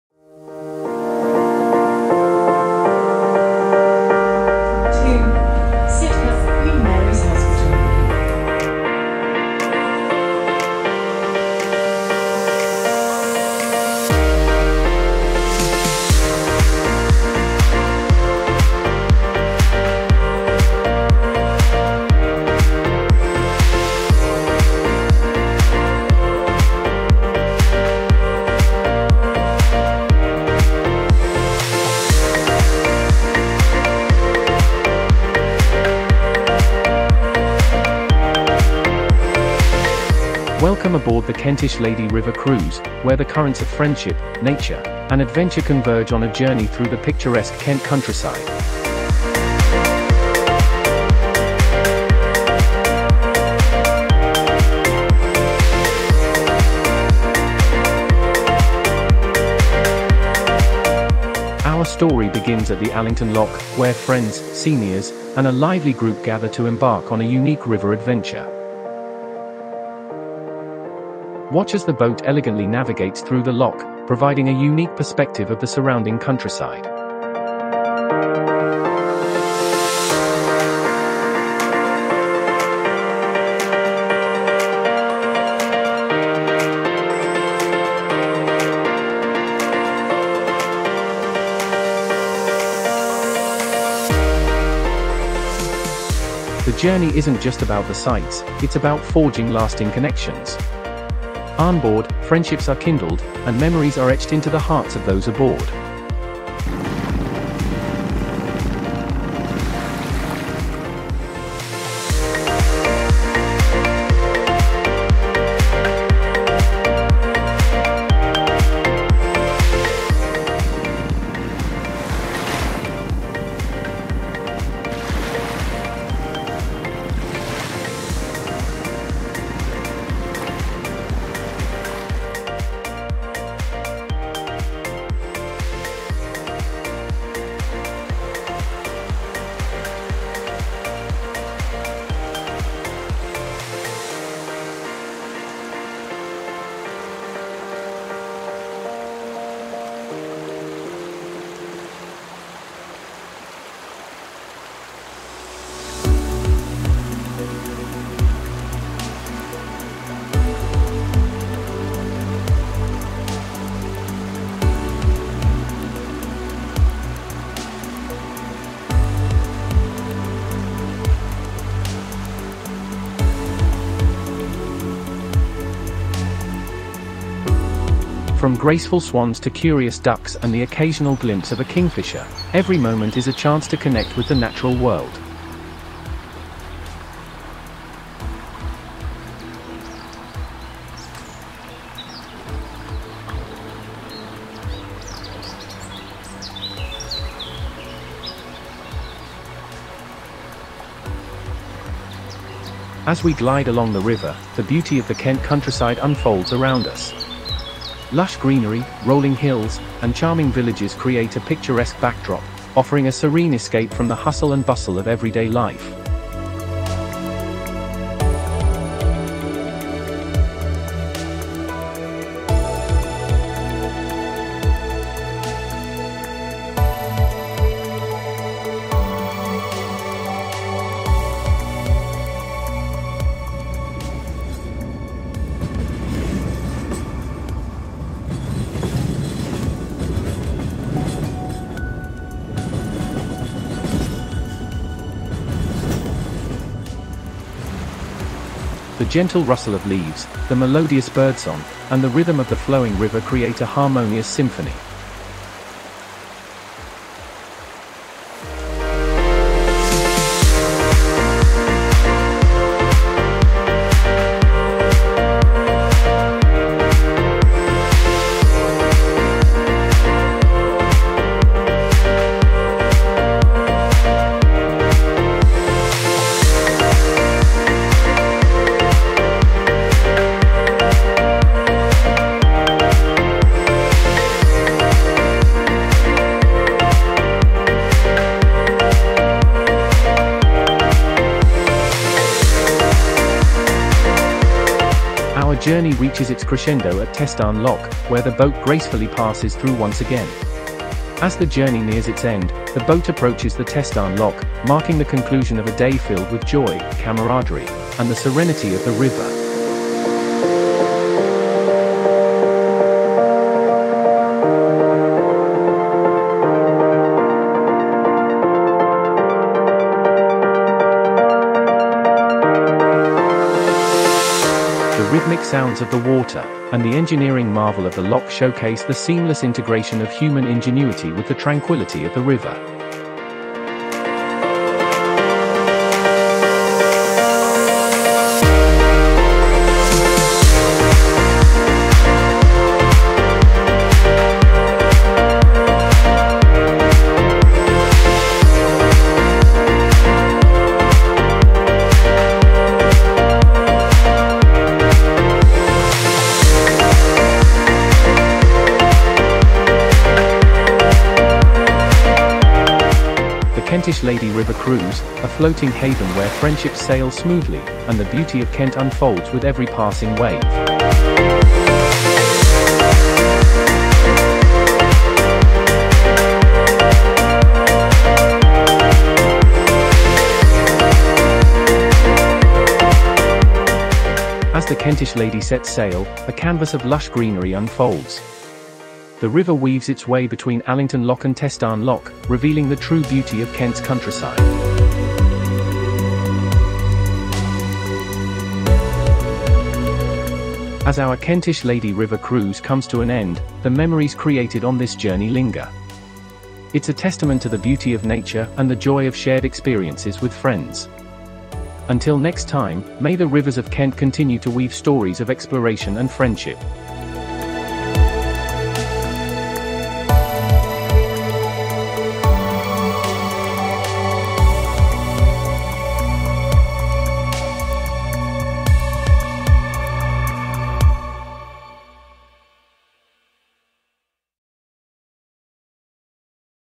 Journey Along the Kentish Lady || Sidcup Speakability Group || 2023
A Short Documentary
"Journey Along the Kentish Lady: A River Cruise through Nature's Canvas"
Videography, Editing & AI Script-Voice: Moinul Hossain Mukul
Acknowledgment: Sidcup Speakability Group
Release Short Documentary: The Christmas party on Tuesday, 19th December, 2023